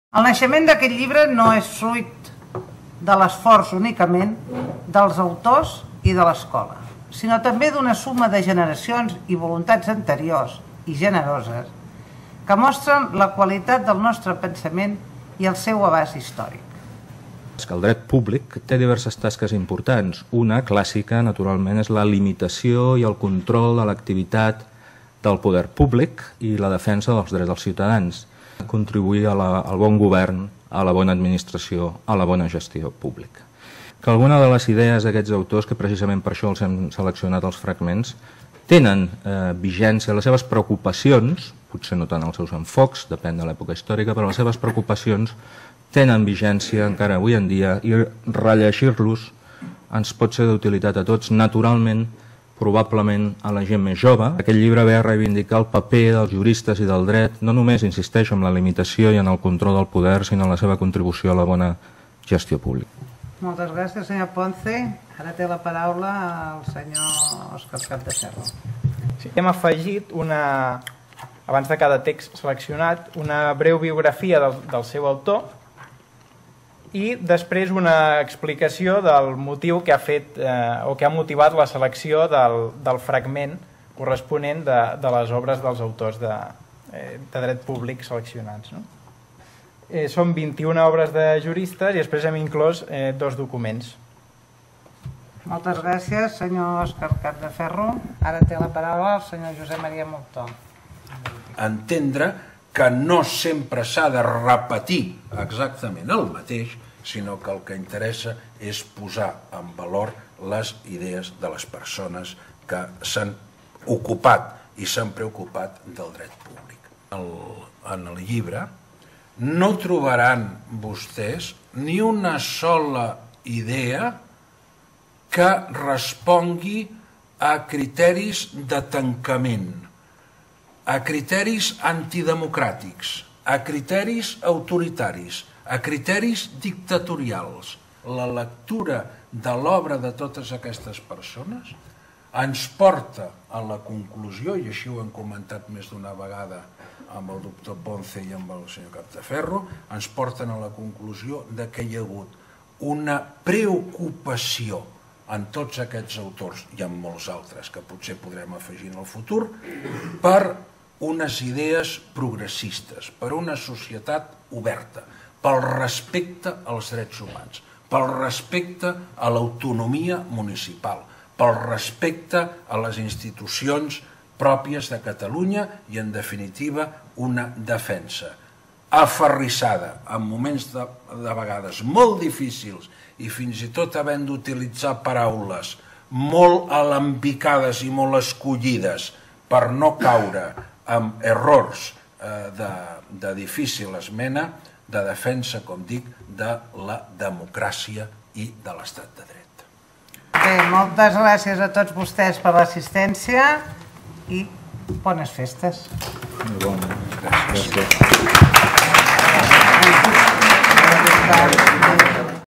Presentació del llibre: "Dret, juristes i gestió pública a Catalunya"
Resum de la presentació del llibre "Dret, juristes i gestió pública a Catalunya a càrrec dels autors de l'obra a l'Escola d'Administració Pública de Catalunya Trobareu més informació de la presentació i el text complert del llibre a